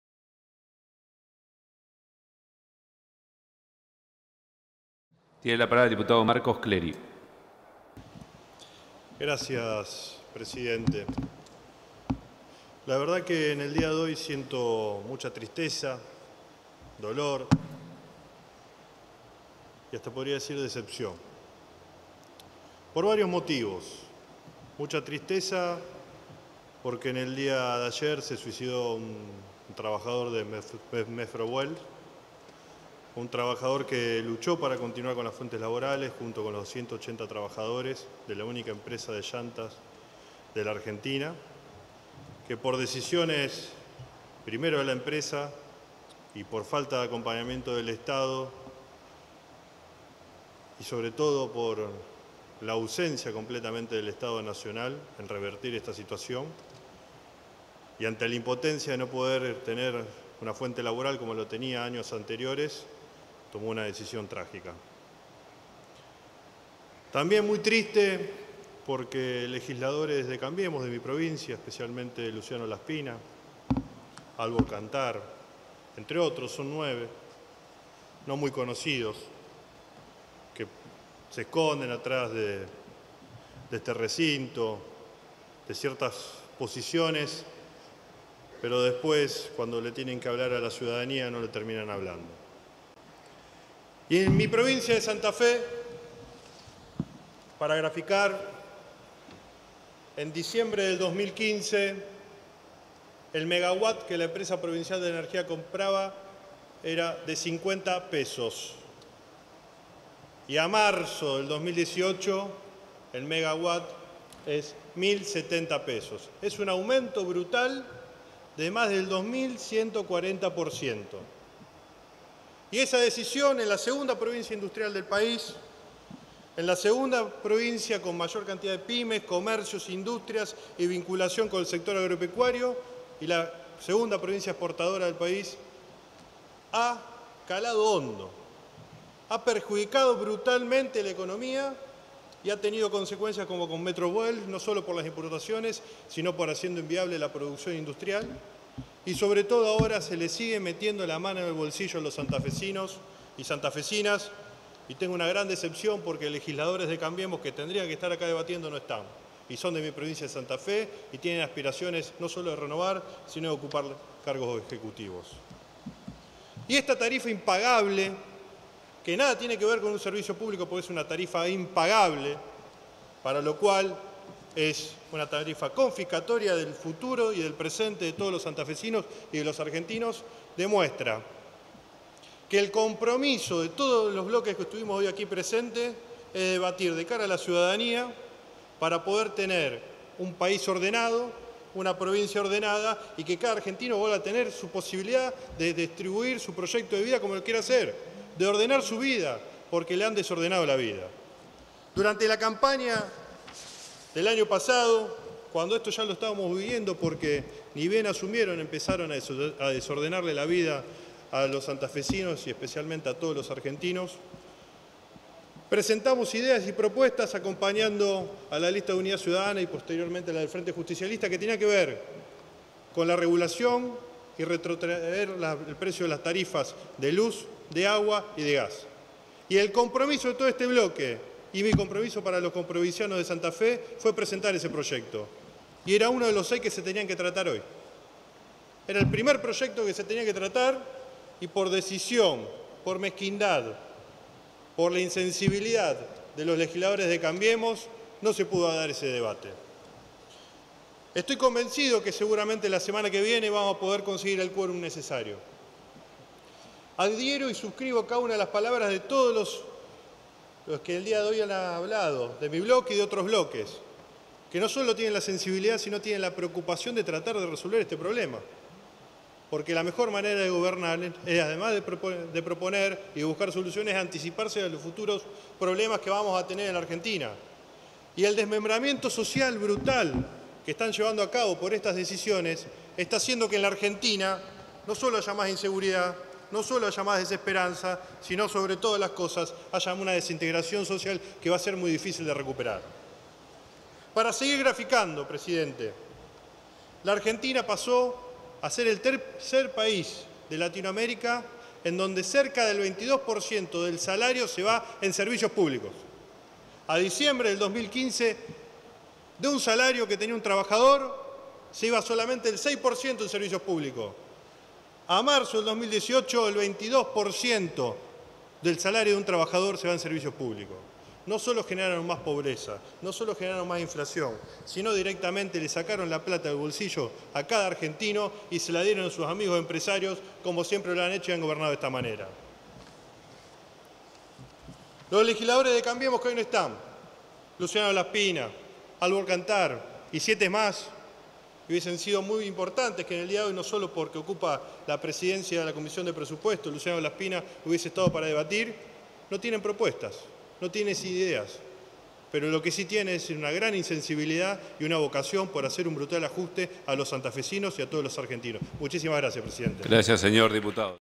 Diputado Cleri Marcos - Expresiones en Minoría 18-04-2018 - EM
SESION ESPECIAL SOLICITADA POR EL SR. DIPUTADO ROSSI Y OTROS SRES. DIPUTADOS

A fin de considerar los siguientes proyectos de ley:
0310-D-18 DE LEY. DEJANSE SIN EFECTO LOS AUMENTOS TARIFARIOS DE LOS SERVICIOS PUBLICOS DE GAS, ELECTRICIDAD, AGUA POTABLE Y CLOACAS, A PARTIR DEL MES DE ENERO DE 2018.

1781-D-18 DE LEY. REVISION DE TARIFAS DE SERVICIOS PUBLICOS.

1761-D-18 DE LEY. CONGELAMIENTO DE TARIFAS DE SERVICIOS PUBLICOS CONCESIONADOS. CREACION DE LA COMISION PARA LA AUDITORIA, BALANCE Y REVISION DE LAS MISMAS.

1714-D-18 AUMENTO DE TARIFAS DE SERVICIOS PUBLICOS. SE DEJAN SIN EFECTO DESDE EL 1° DE ENERO DE 2016.

869-D-17 DECLARASE LA EMERGENCIA EN EL ACCESO A SERVICIOS PUBLICOS ESENCIALES - SUSPENSION DE AUMENTO DE TARIFAS. CREASE LA COMISION BICAMERAL DE EVALUACION DEL REGIMEN TARIFARIO.

1685-D-18 ACCESO IGUALITARIO A SERVICIOS ESENCIALES Y BIENES PUBLICOS. REGIMEN.